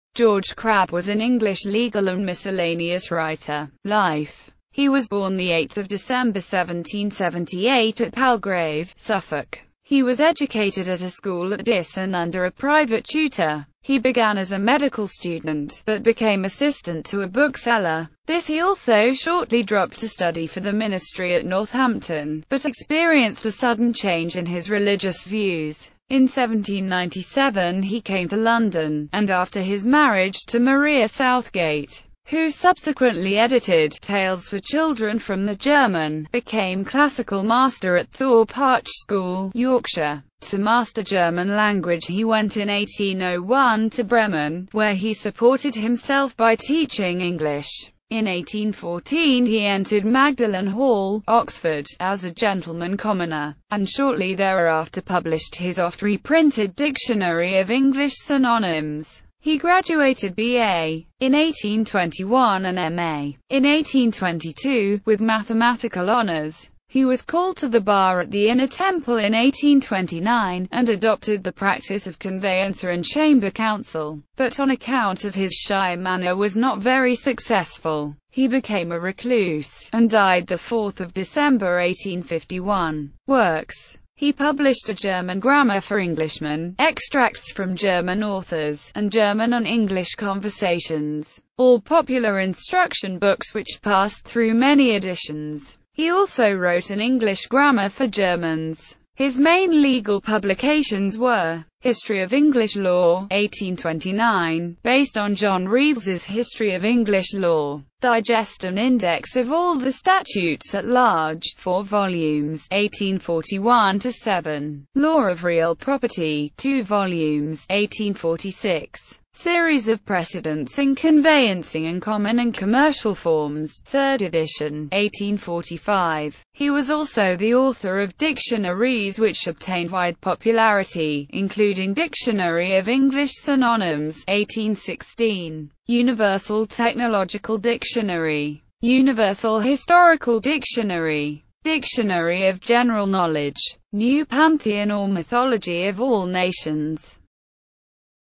George Crabb (writer)
George Crabb (1778–1851) was an English legal and miscellaneous writer.

Image-Copyright-Info
Image is in public domain
Author-Info:   George Crabb (Universal Historical Dictionary)
Image-Copyright-Info